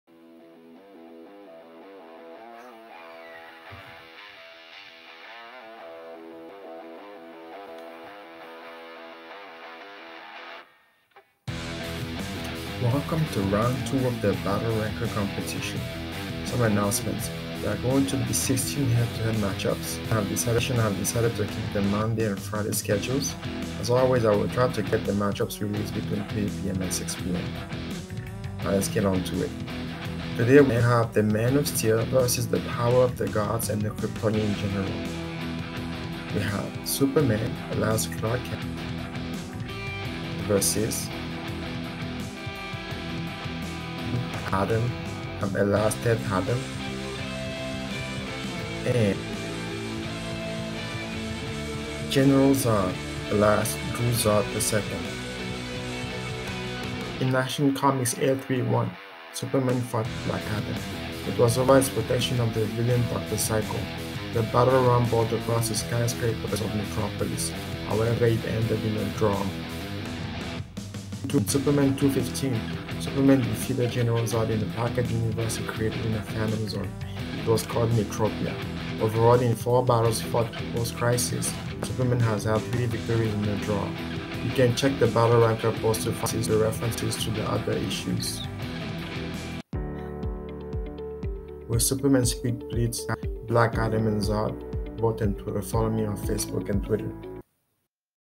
Battle Ranker Round 2: Superman vs Black Adam And Zod
Welcome to the Battle Ranker Competition,

A year-long melee battle between 100 DC and Marvel characters.

In Round 2, Number 1 ranked Superman is challenged by Number 8 ranked Black Adam and Number 29 ranked Zod.